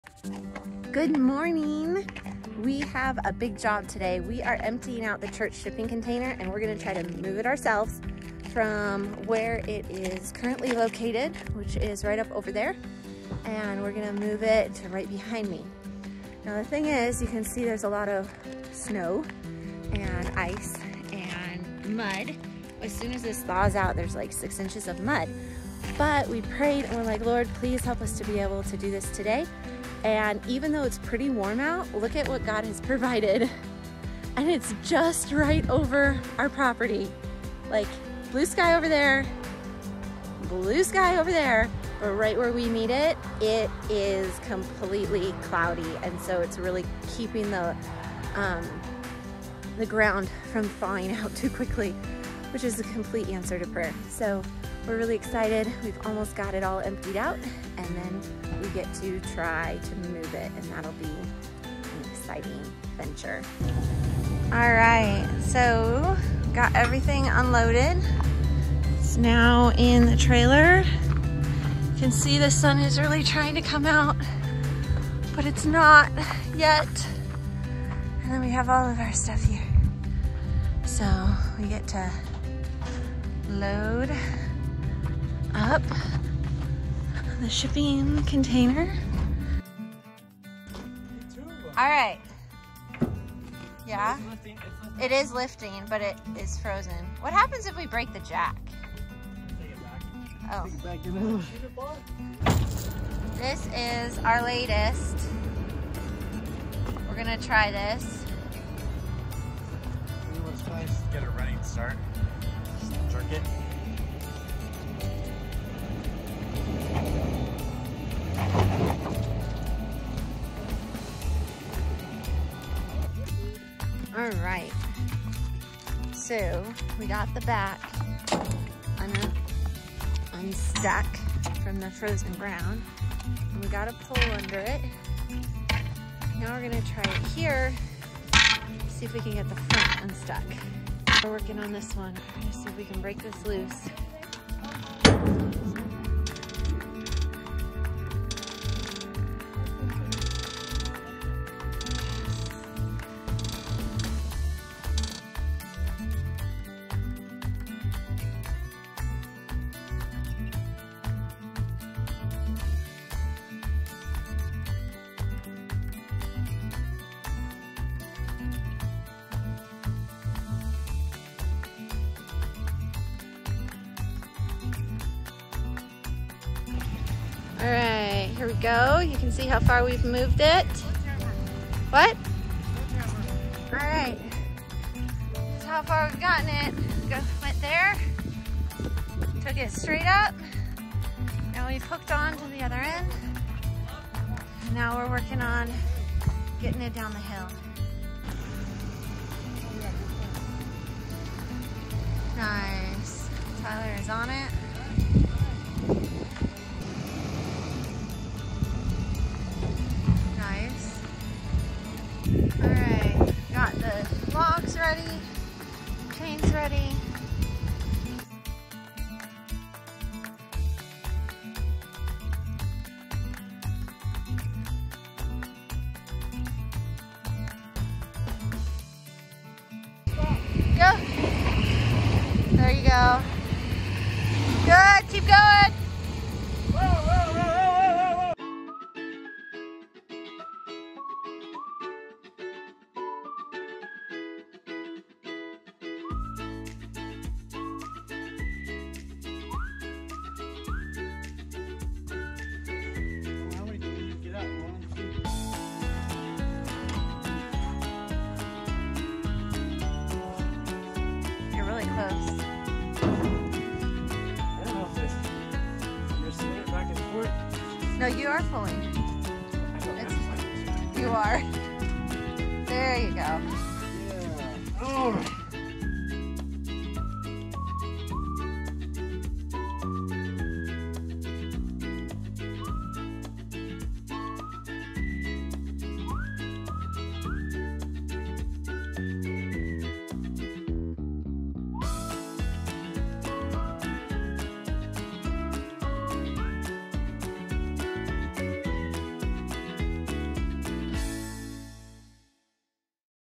Moving the Shipping Container
In order to start on our next project of a "Tiny House", both 40ft shipping containers NEED to be moved! But it was going to cost a LOT of $$$$. So, we decided to try and move it ourselves. Join us as we use trucks, fence posts, chains, a floor jack, and a whole lot of determination to get the shipping container to JUST the right place!